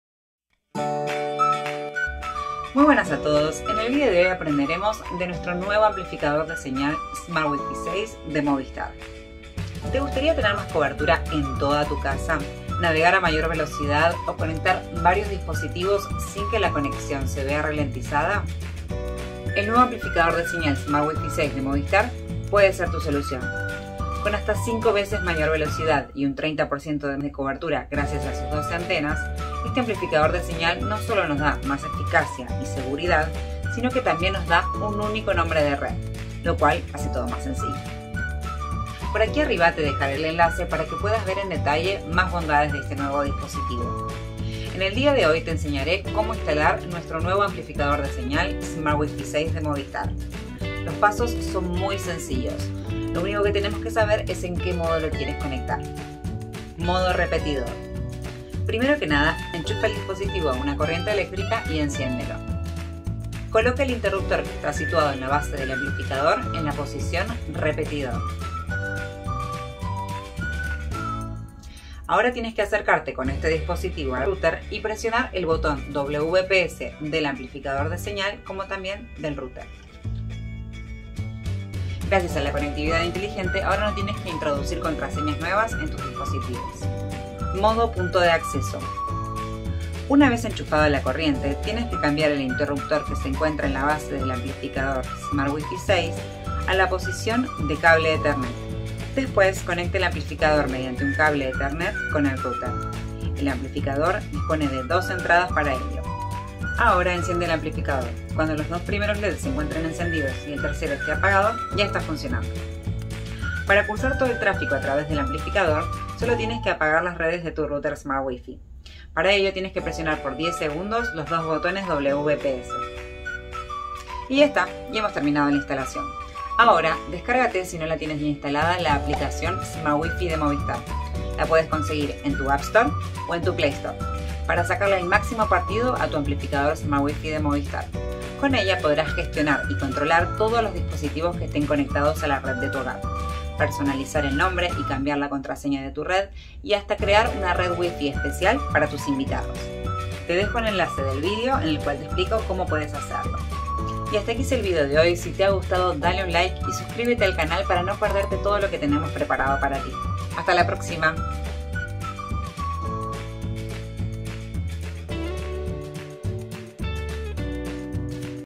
MOVISTAR: Cómo instalar el amplificador Smart WiFi 6
El amplificador Smart WiFi 6 de Movistar se puede instalar y configurar como amplificador o bien como punto de acceso. Te explicamos cómo en este vídeo.